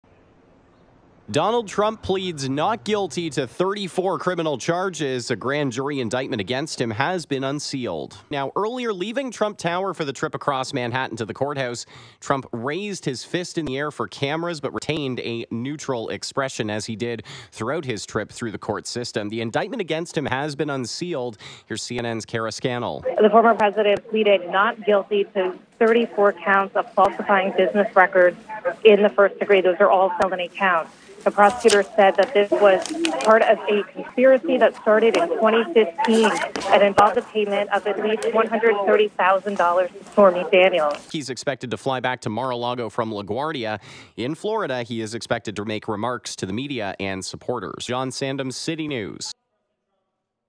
Former President Donald Trump pleads not guilty to falsifying business records
Donald Trump appeared in a New York courthouse this afternoon, pleading not guilty to 34 counts of falsifying business records.